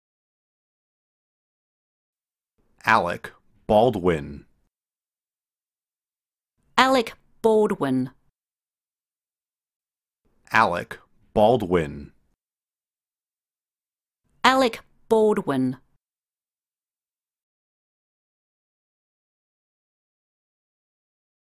American vs Australian Accent: How to Pronounce ALEC BALDWIN in an Australian or American Accent
Today we are learning to pronounce the word ALEC BALDWIN